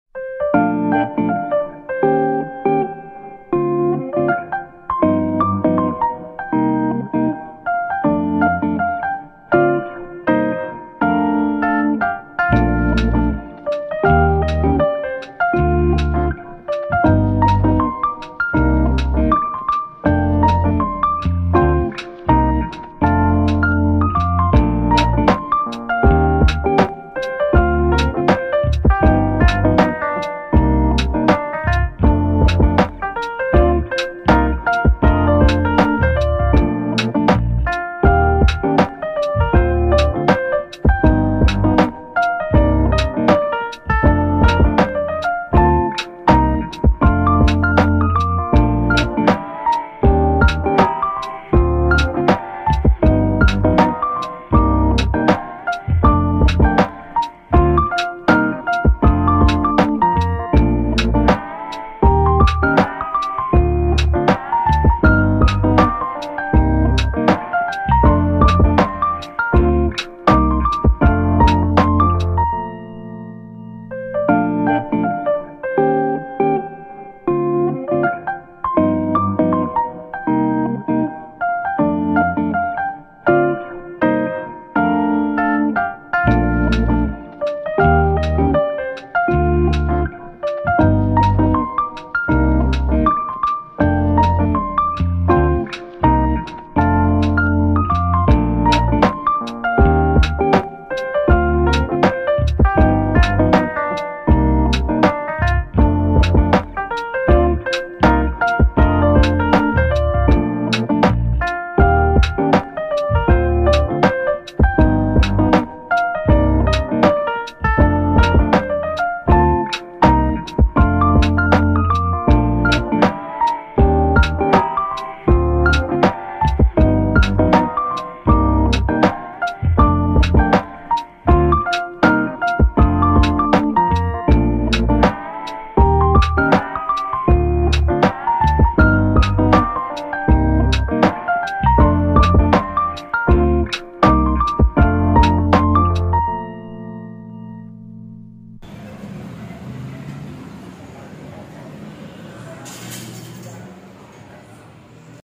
One more unusual Bangkok cafe: Bakers Bridge cafe
Bakers Bridge cafe in Saphan Kwai has a great old style decoration resembling more of a museum than a coffee shop